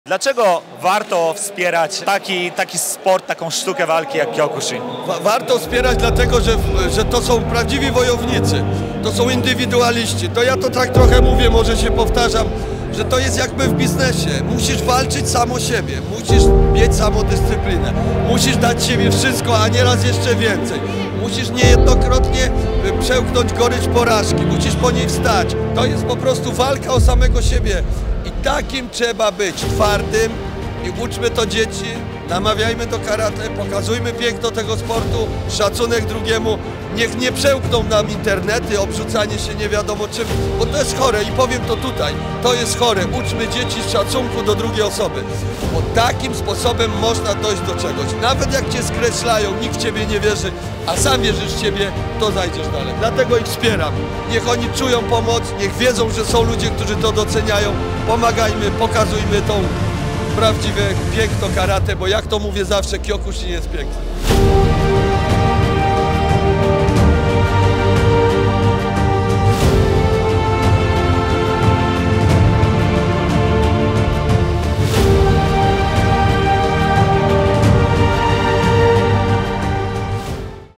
Dlaczego warto wspierać Karate Kyokushin?
Krótka, ale jakże treściwa wypowiedź jednego z właścicieli firmy N.O.W.I., który w pięknych słowach tłumaczy dlaczego warto wspierać karateków, dlaczego warto trenować Karate Kyokushin!

Andrzeju, wielkie OSU!
DOMO ARIGATO!